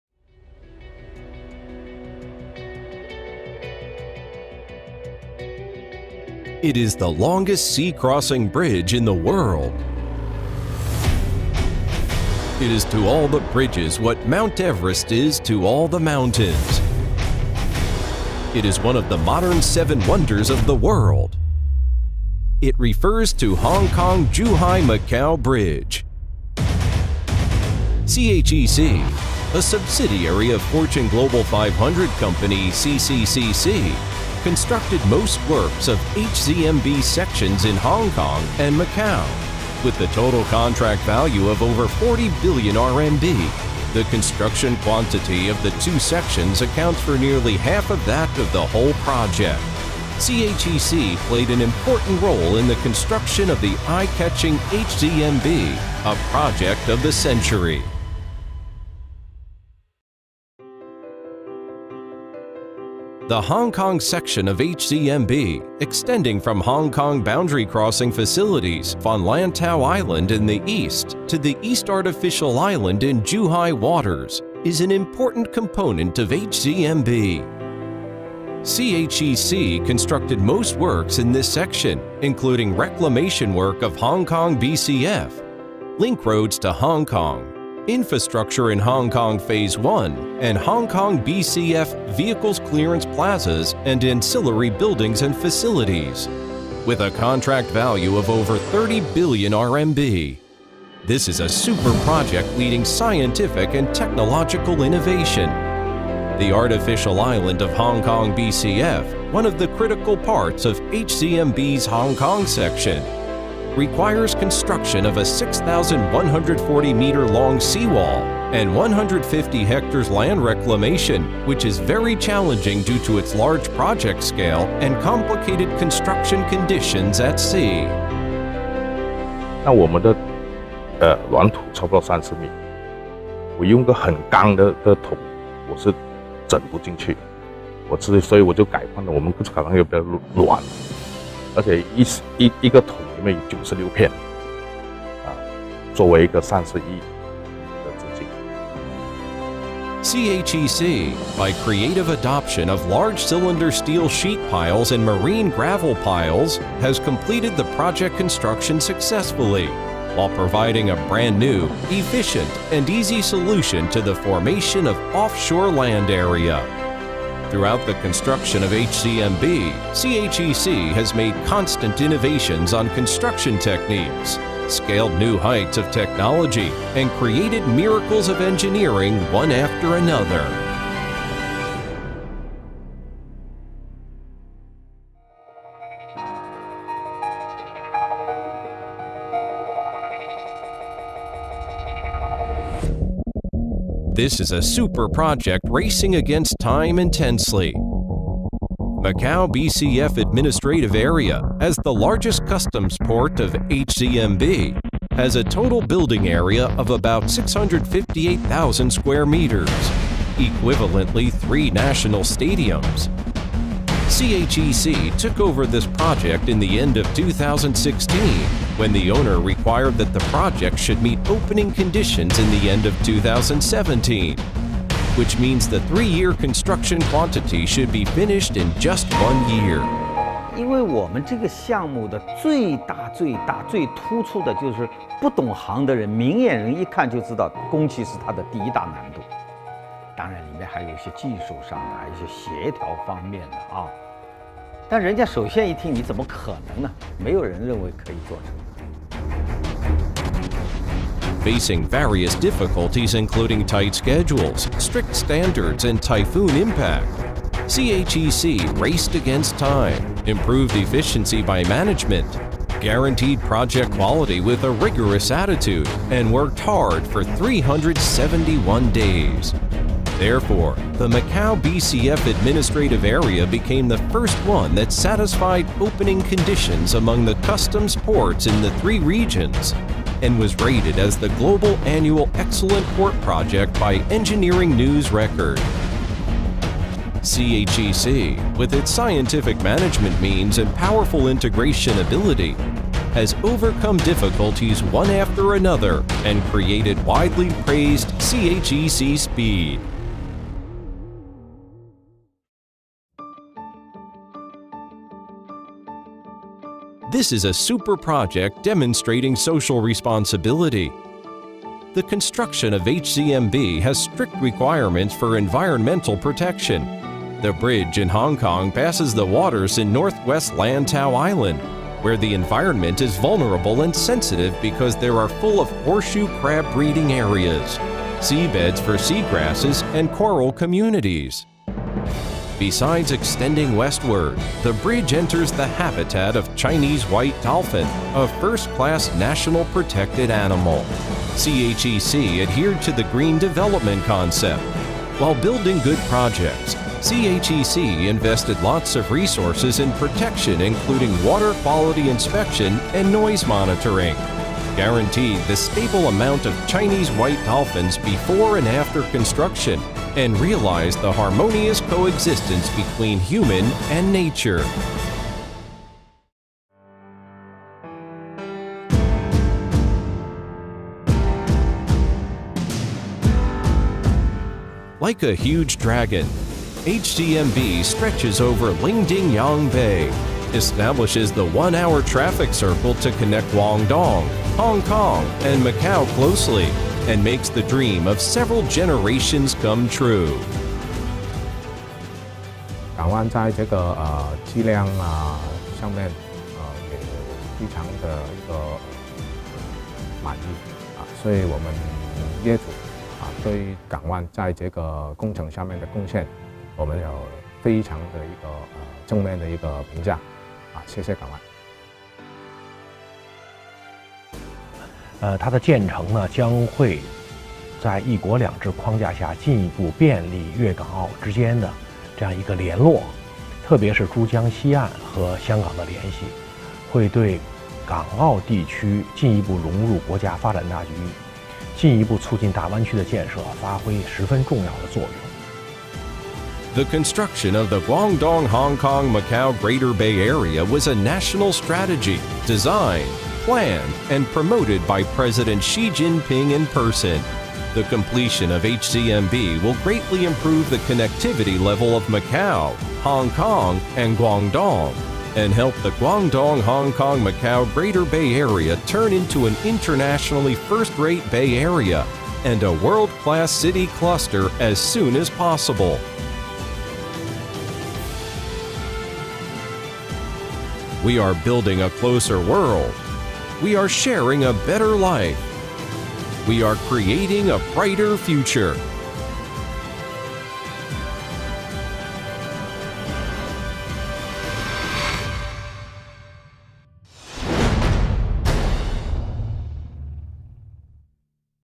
"HZMB Macau Port" with 600,000 square meters built in 371 days, main contractor：China Harbor (CHEC)
Hong Kong-Zhuhai-Macau Bridge (hereinafter abbreviated as HKZMB) Macau Port locates close together with its counterpart HKZMB Zhuhai Port on the frontier port artificial island. It consists of the Frontier Post, the East and the West Car Parks as well as auxiliary services & facilities.